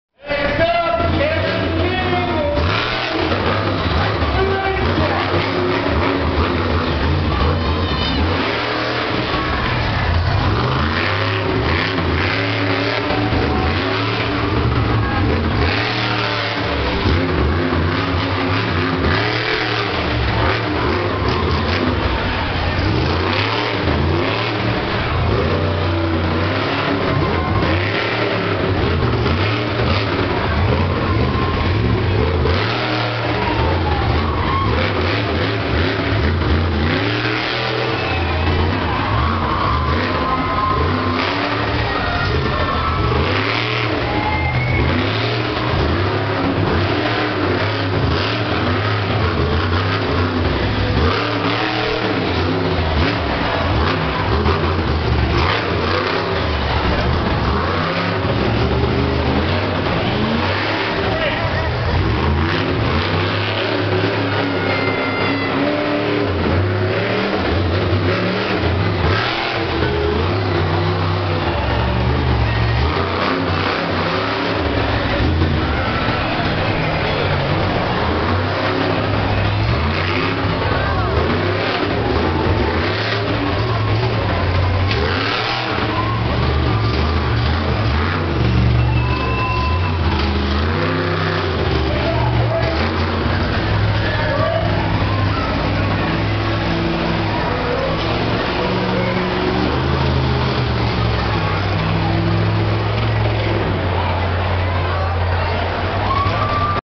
Ultimate Mega Monster Truck Championships Monroe County Fair 2021: Strait Jacket Freestyle
ATR Motorsports Promotions
Ultimate Mega Monster Truck Championships at Monroe County Fair
Monster Truck Freestyle
Kevin King in Strait Jacket